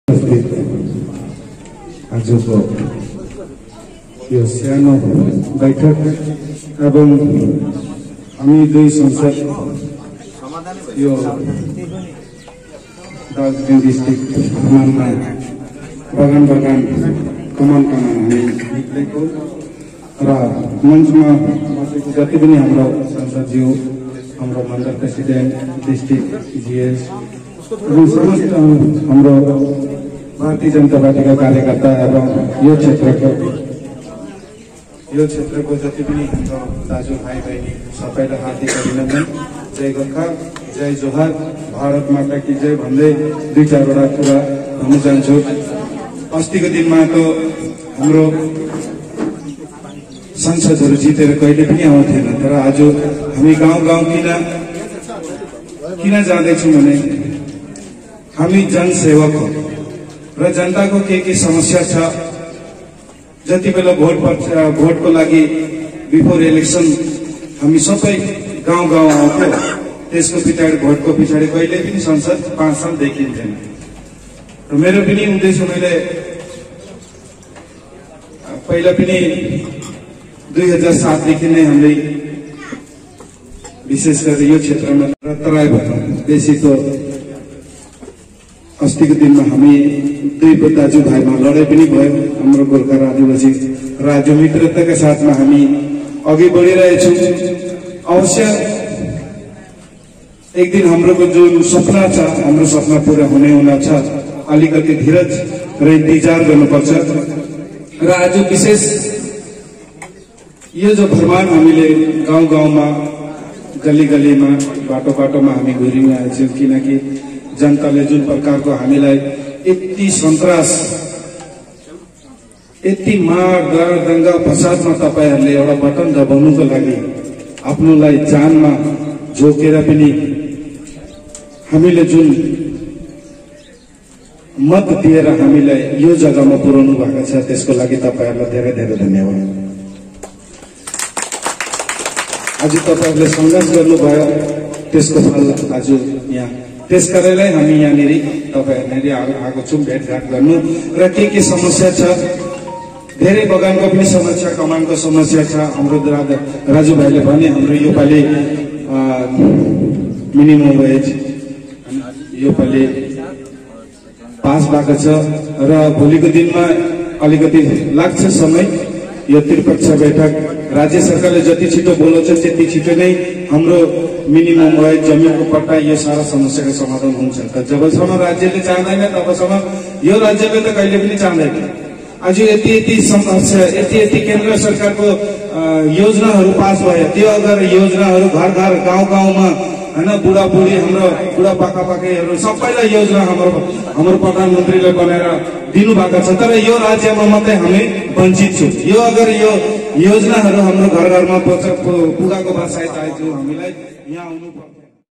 Alipurdwar MP John Barla giving information about kisan bill and minimum wages plan.
Alipurdwar MP John Barla giving information about kisan bill and minimum wages plan wich are recently passed at parliament.